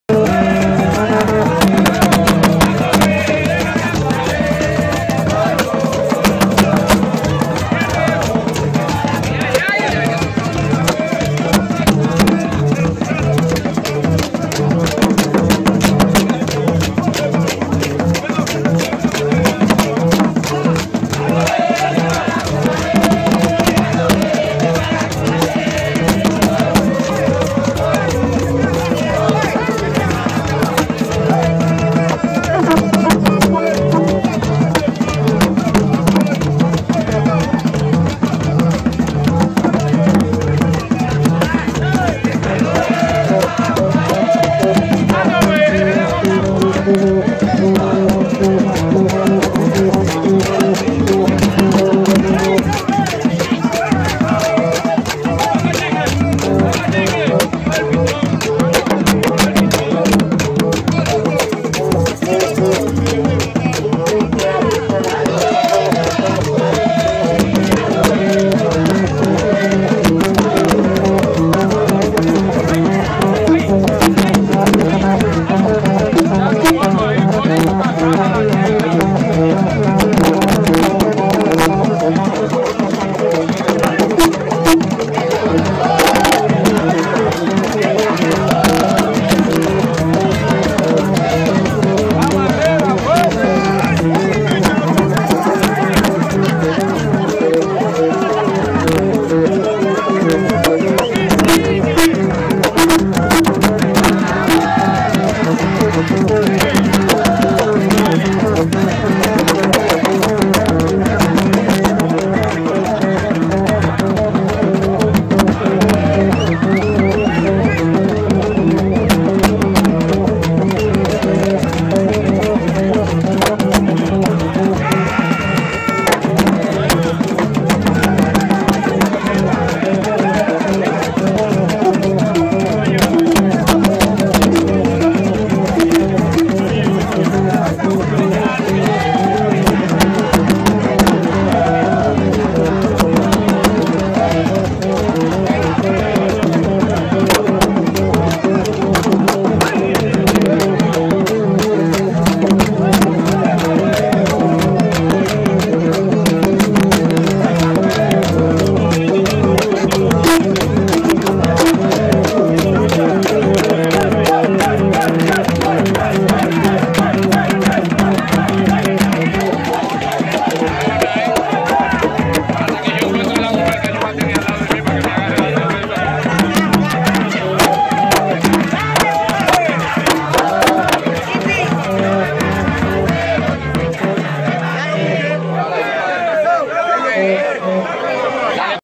GAGA DE BERMEJO -ADORANDO LA SANTA CRUZ 23-02-2019
MOVIMIENTO GUETTO GAGA GRUPO CULTURAL RELIGOSO.. Nuestra primera práctica del 2019 todo un éxito..